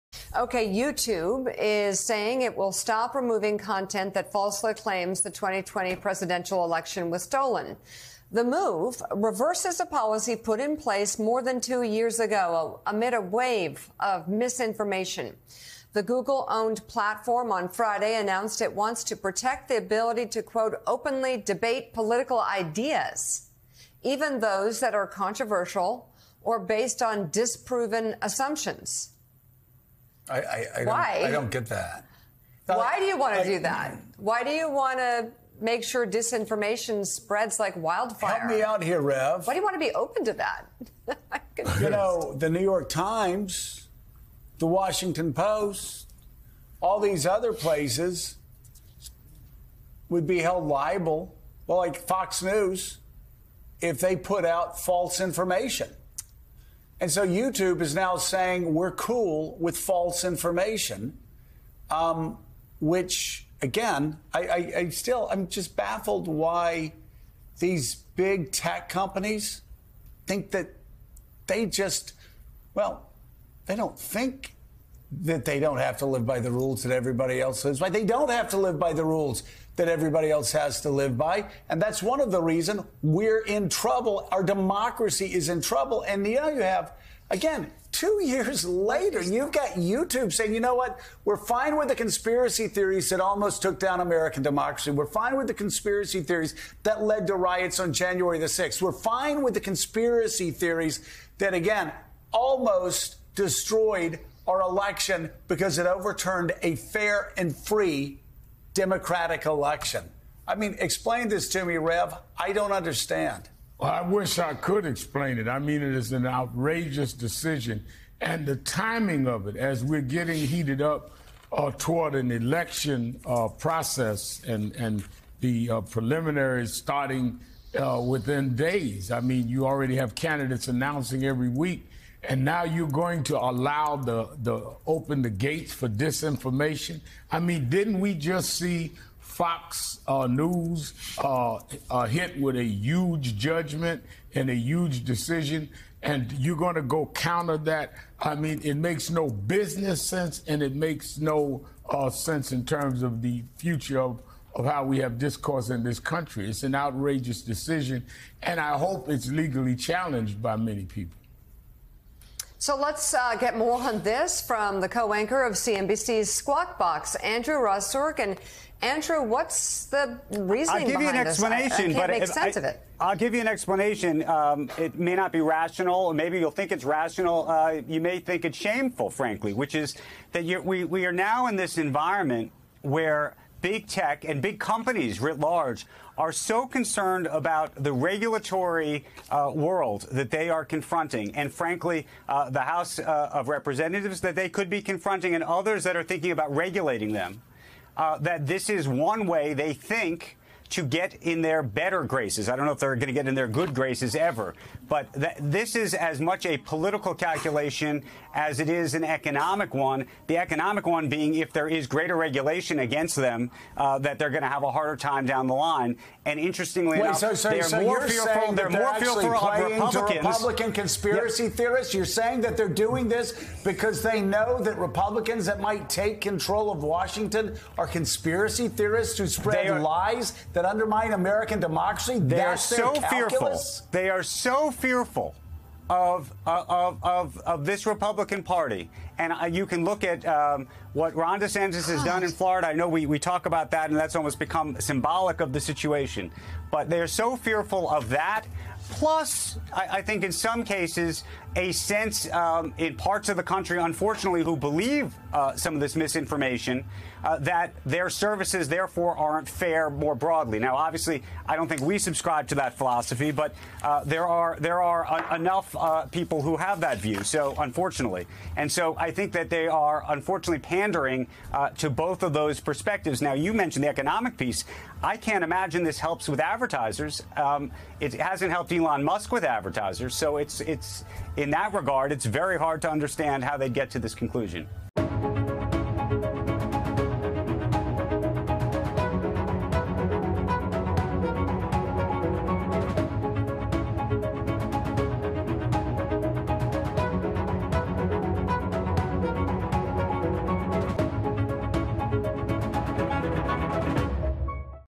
YouTube to again allow false claims about 2020 election fraud
In a reversal of its election integrity policy, YouTube will leave up content that says fraud, errors or glitches occurred in the 2020 presidential election and other U.S. elections, according to reports.